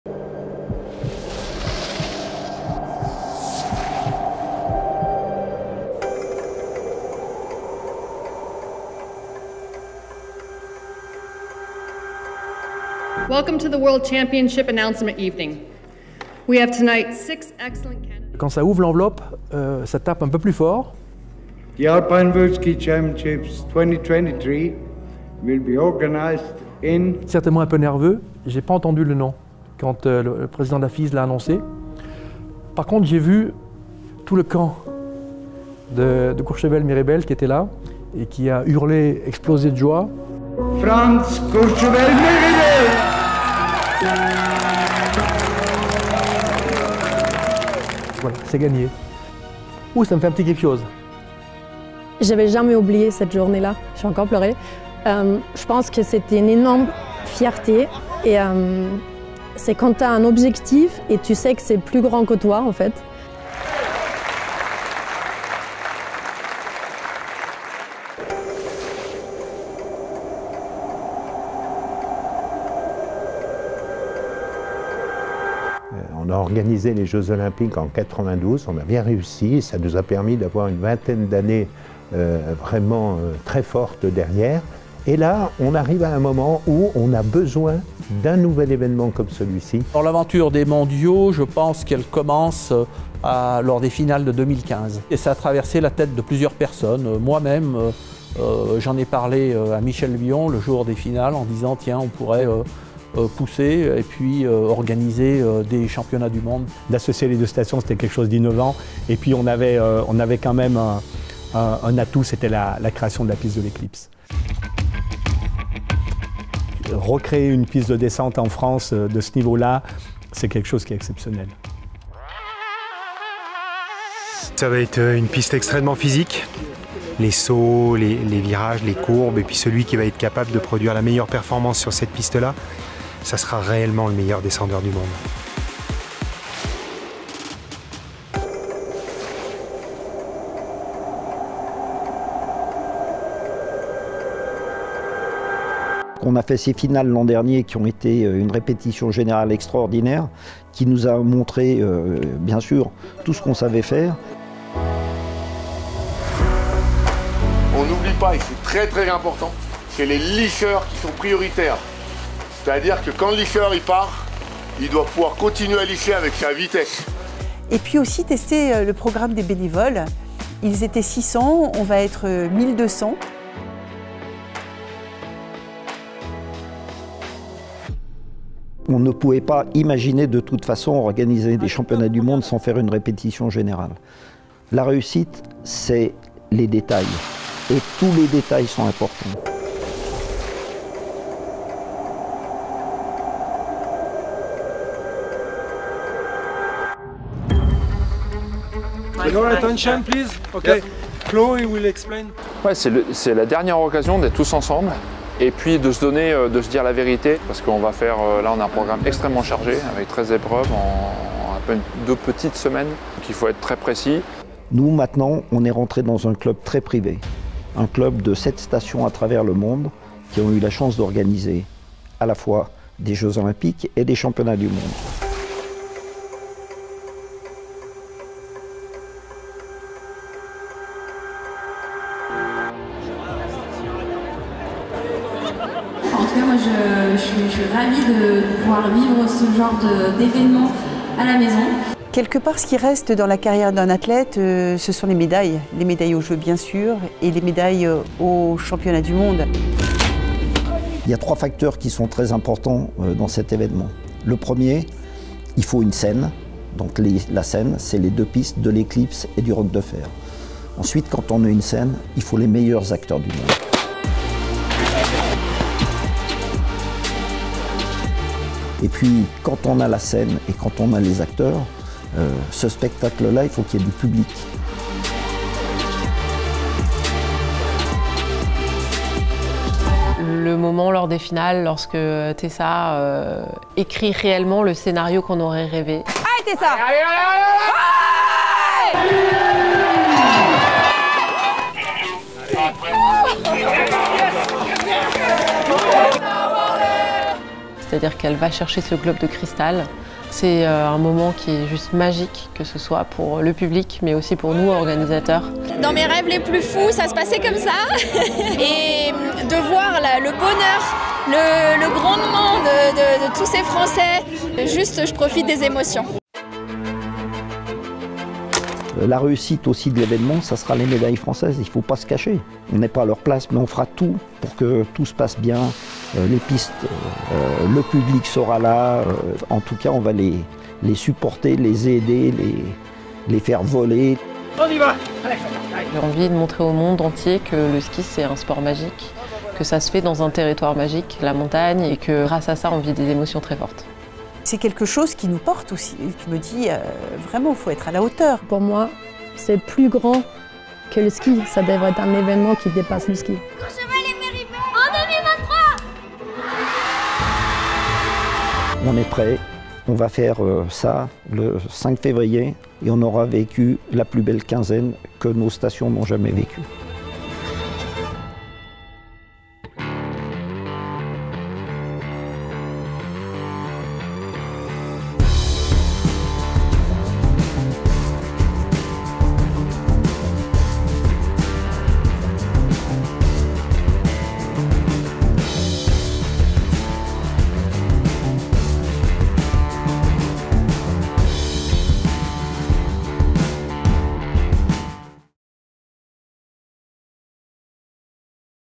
"A l'unisson": de la candidature à l'événement, la belle histoire des Mondiaux de Courchevel-Méribel
Les championnats du monde de ski alpin sont organisés en France pour la 4e fois seulement. Après Chamonix, Val d’Isère, les stations de Courchevel Méribel seront au centre du monde dès dimanche 5 février. Une double candidature pour un projet unique, une première portée par des femmes et des hommes. Il aura en effet fallu 8 ans pour que cette idée née dans l’esprit de quelques-uns se transforme en rendez-vous international. De Costa Navarino à la création de la piste de l’Eclipse en passant par les inspections de la FIS et la grande répétition des finales de la Coupe du monde, ces passionnés de ski nous font revivre ces instants à quelques jours de l’ouverture des Mondiaux...
Réalisation: Sébastien Géroudet / PixPocket / Courchevel-Méribel 2023